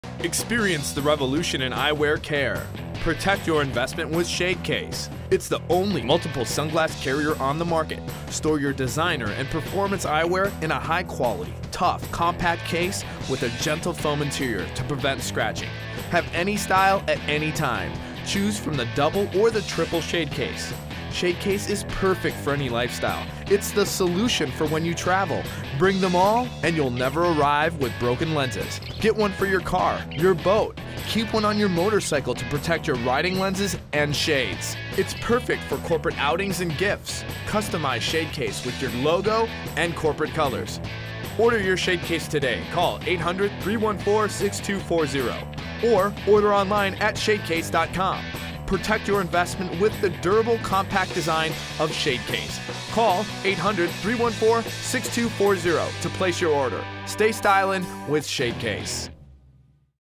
Shadecase
Protect your glasses with this durable storage holder.